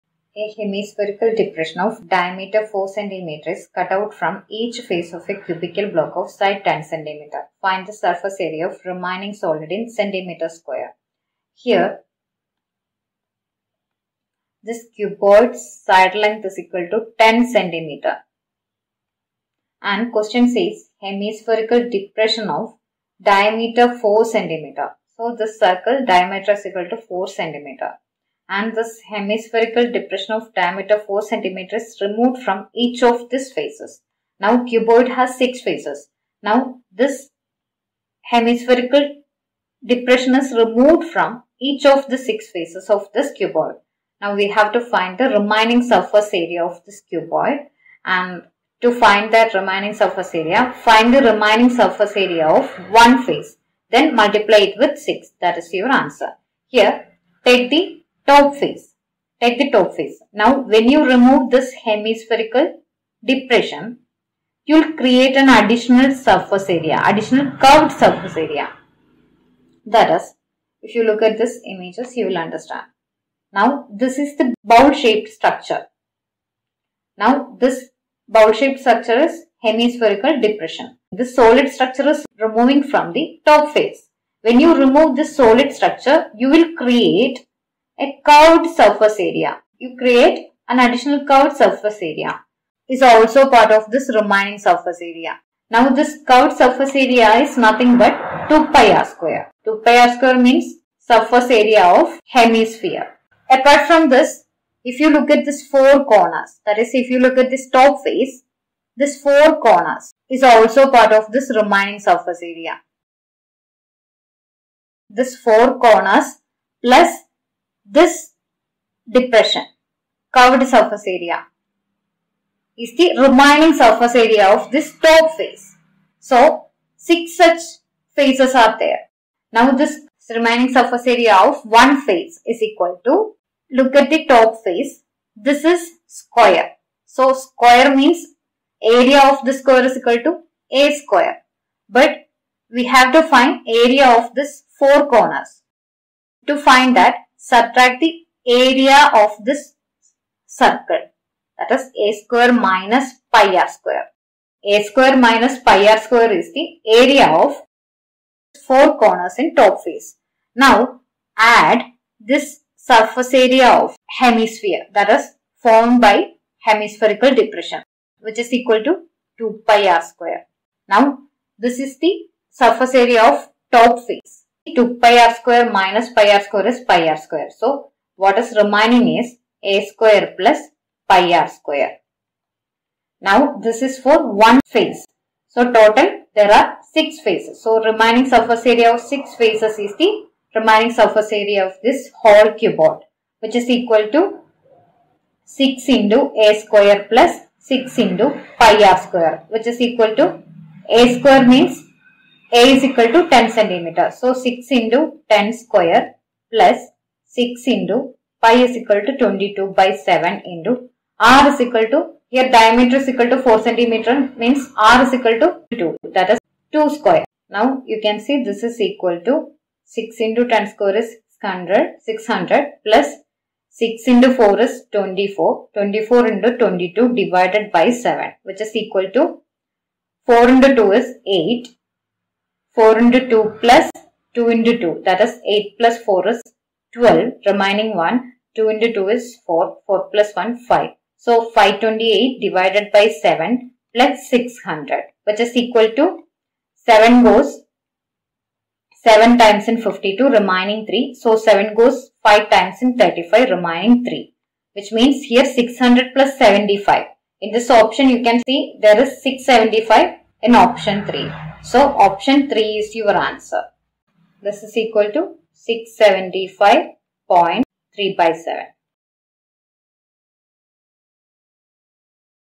A hemispherical depression of diameter 4 cm is cut out from each face of a cubical block of sides 10
cgl previous year question paper tier 1

00:00 A hemispherical depression of diameter 4 cm is cut out from each face of a cubical block of sides 10 cm. Find the surface area of the remaining solid (in cm² ).